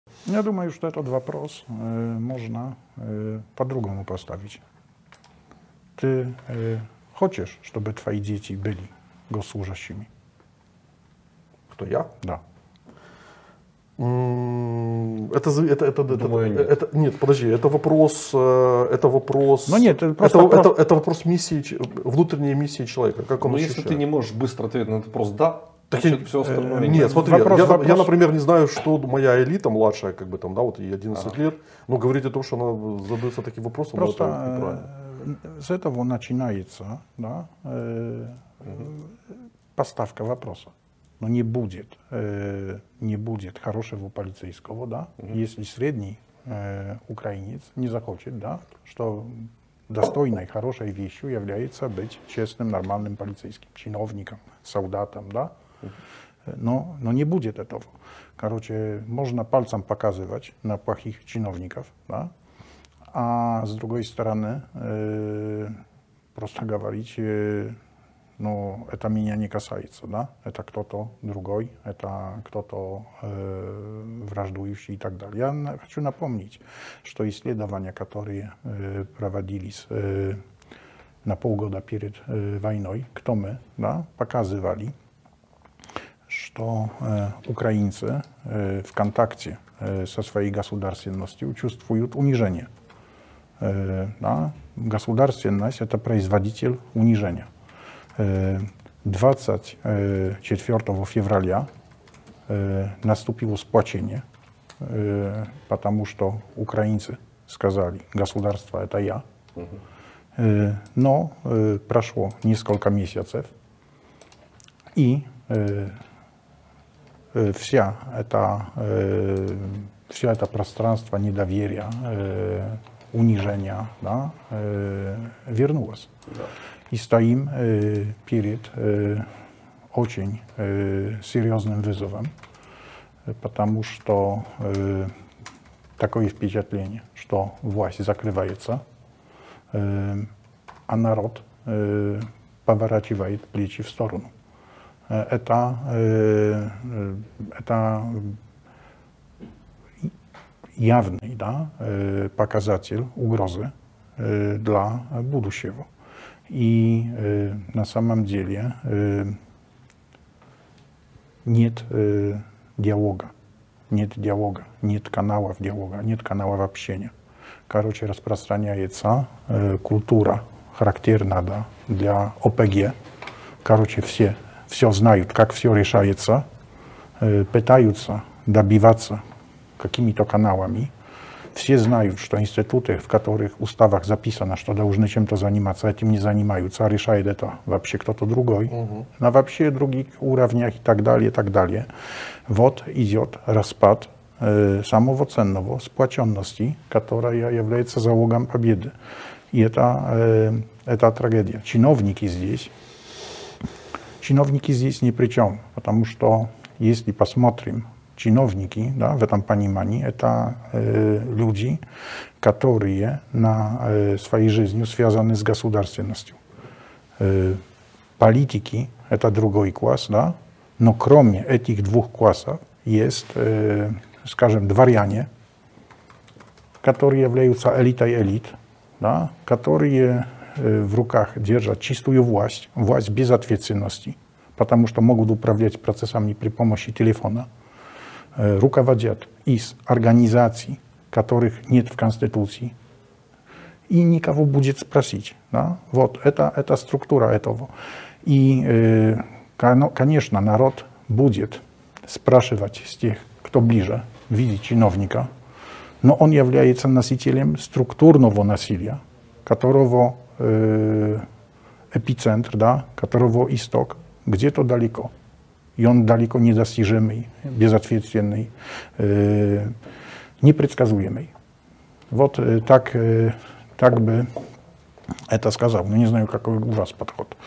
Идет распад сплоченности украинцев. Государство - источник унижения народа. Пьотр Кульпа
Государственность в Украине - источник унижения народа. Война сплотила народ. Народ сказал: “Теперь я государство!”. Сейчас наблюдается обратные тенденции, которые разрушают сплоченность украинцев. Пьотр Кульпа